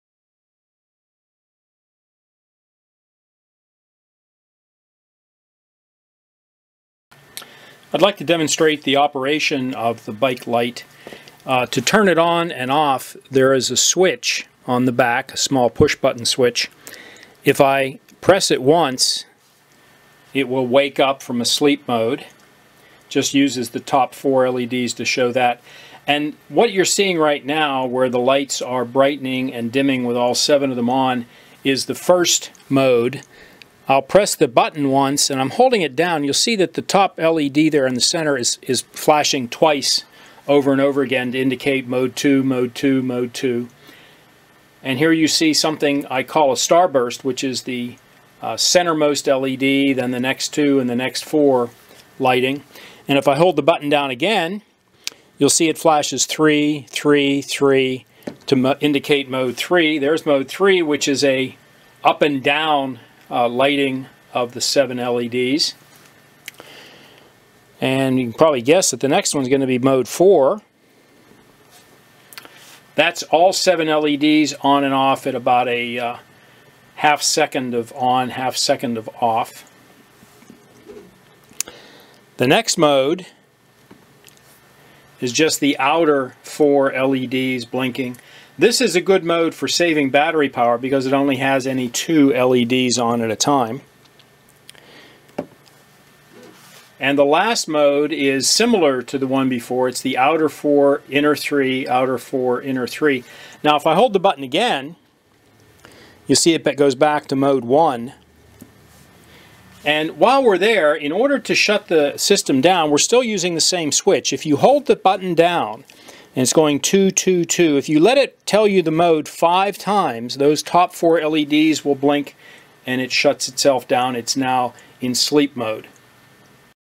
7 LED Bike Blinker with 3D Printed Case
This rear bike light has seven individually addressable LEDs that can be used to make for safer bike riding in the dark.
Six different modes can be chosen by pressing a single button.
The case is made with a 3D printer.  It and the software can be modified to meet your needs.

Note: the video is shot at a steep angle to keep the VERY bright LEDs from blinding the camera.